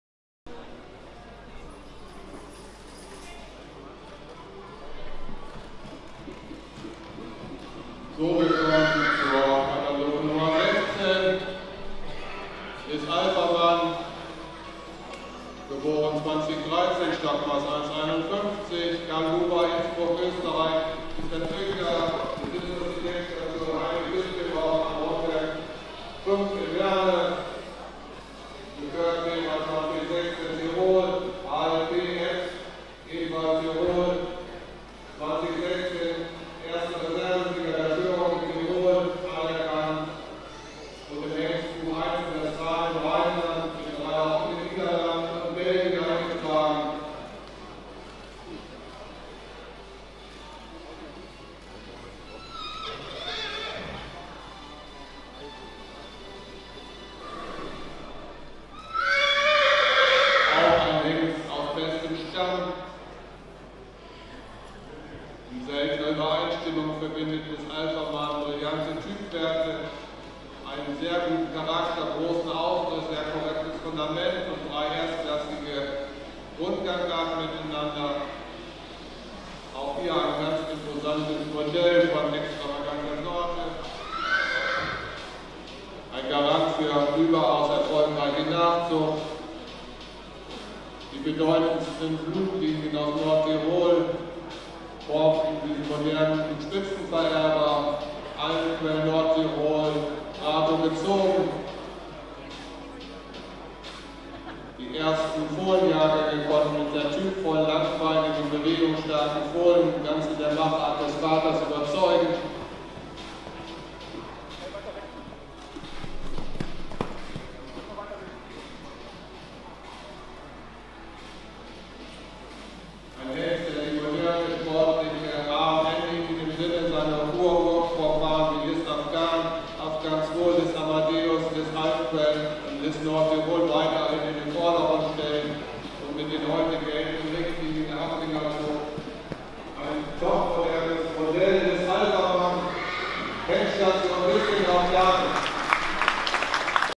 16 liz Alvermann an der Hand
10. OHD Hengstpräsentation der Haflinger
in Münster Handorf
am 03. März 2019

Fotos & Video sind erhältlich bei
Rainer Kohl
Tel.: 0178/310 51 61

Kopieren und Vervielfältigen ohne Genehmigung ist untersagt.